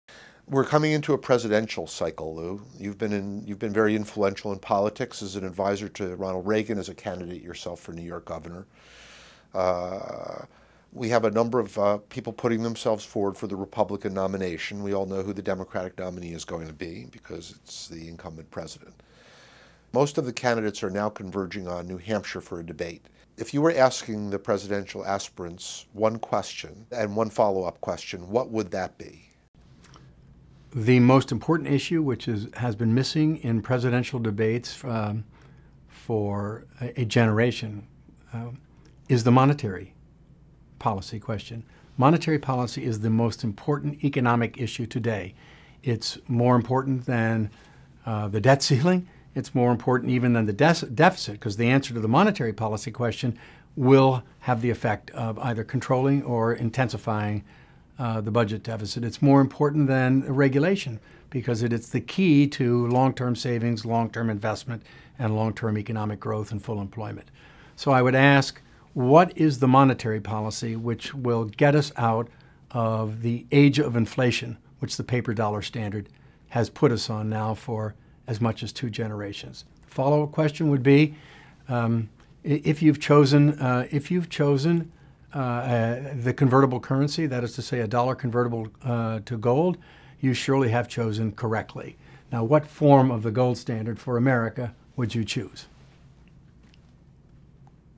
GOP 2012 Candidates: What is your Monetary Policy?
Lewis E. Lehrman, Chairman of The Gold Standard Now, poses two questions to the GOP 2012 Presidential Candidates - What is the monetary policy that will get us out of the Age of Inflation? If you have chosen a convertible currency, a dollar convertible to gold, then what form of the gold standard would you choose?